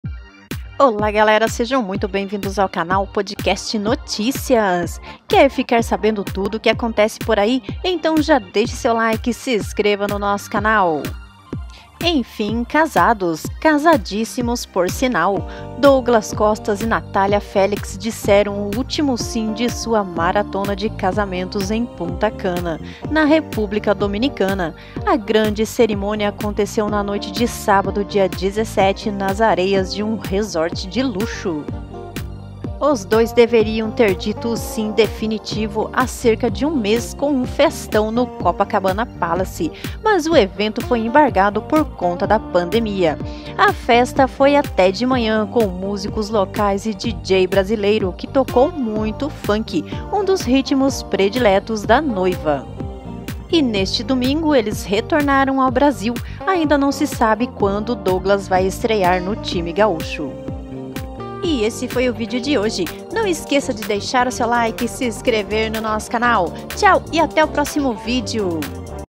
Douglas Costa e Nathalia Félix se Casam em Praia do Caribe
A festa foi até de manhã, com músicos locais e DJ brasileiro, que tocou muito funk, um dos ritmos prediletos da noiva.